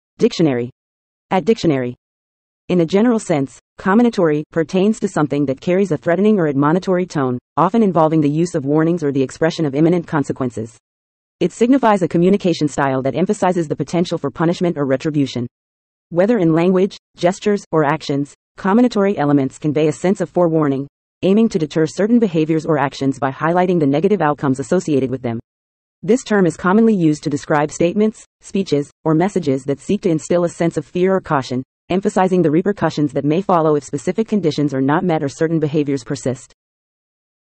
Comminatory Meaning In English
In a general sense, "comminatory" pertains to something that carries a threatening or admonitory tone, often involving the use of warnings or the expression of imminent consequences. It signifies a communication style that emphasizes the potential for punishment or retribution. Whether in language, gestures, or actions, comminatory elements convey a sense of forewarning, aiming to deter certain behaviors or actions by highlighting the negative outcomes associated with them. This term is commonly used to describe statements, speeches, or messages that seek to instill a sense of fear or caution, emphasizing the repercussions that may follow if specific conditions are not met or certain behaviors persist.